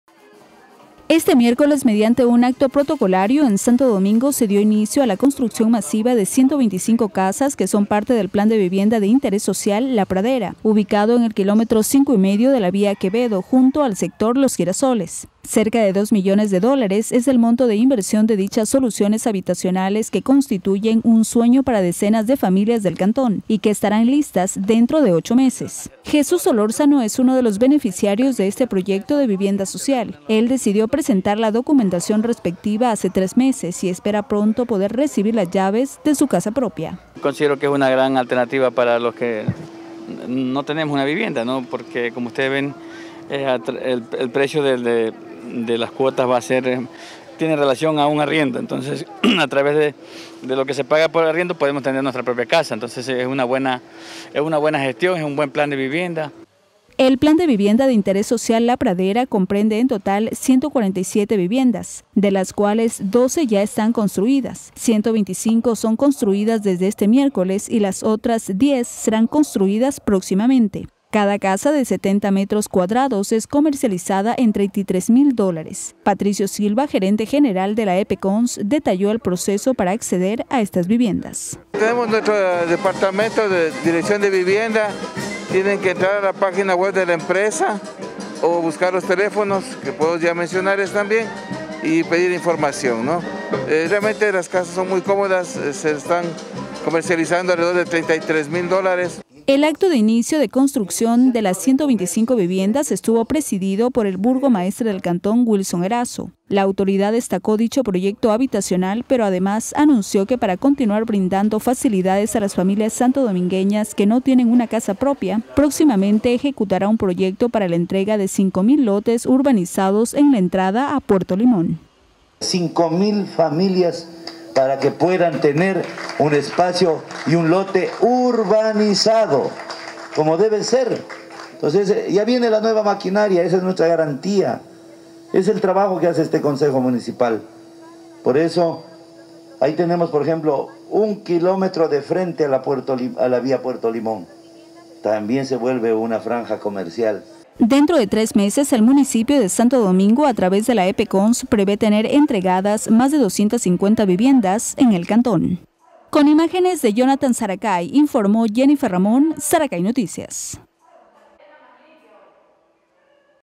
Inició la construcción de viviendas en la Pradera | Noticias | Zaracay TV
La Alcaldía de Santo Domingo a través de EP-CONST inició de manera formal la construcción masiva de 125 viviendas más en el plan habitacional La Pradera, intervención que complementará las 147 viviendas de este proyecto urbanístico.